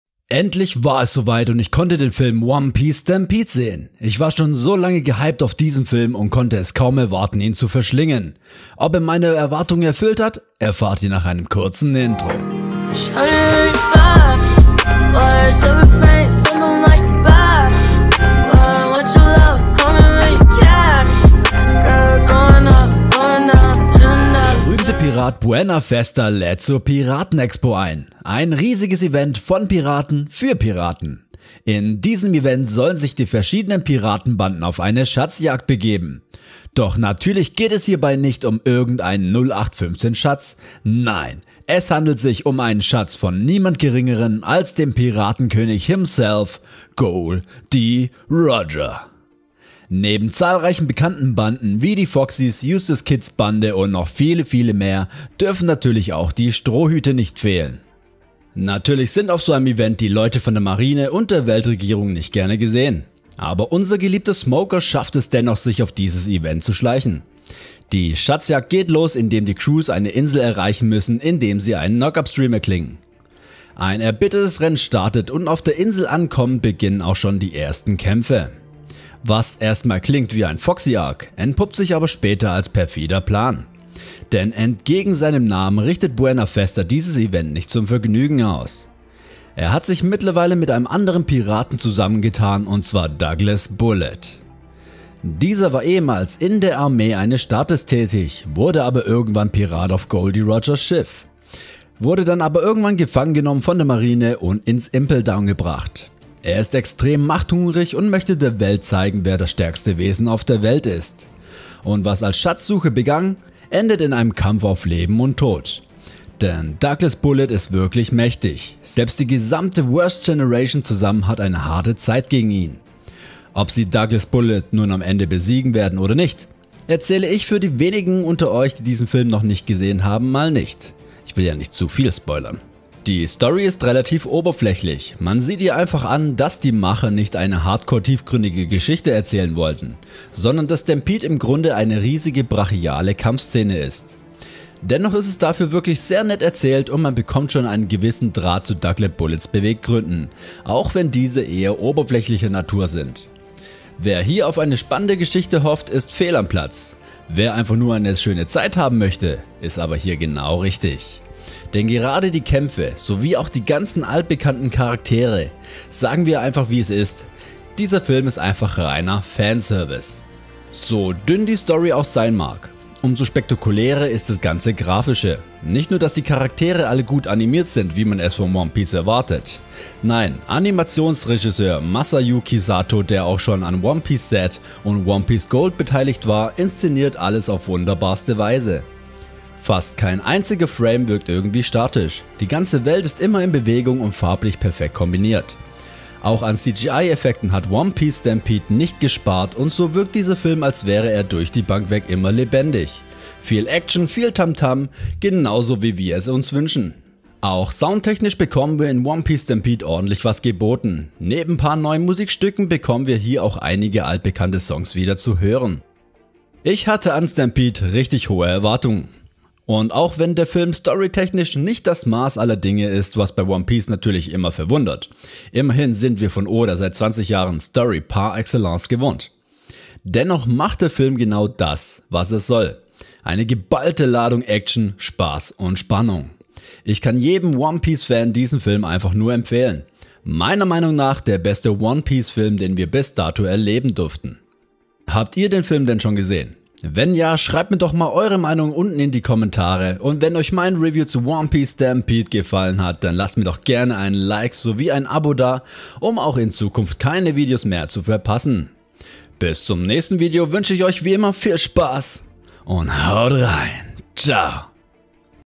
One Piece Stampede Review | Anime Review
Mein Review zum neuen  One Piece Film One Piece Stampede mit Endgegner Douglas Bullet und seine krasse Vergangenheit.

«ONE PIECE»
Eiichiro Oda
Shueisha, 1997
Toei Animation, 1999

MEINE LIVESTREAMS

RECHTLICHES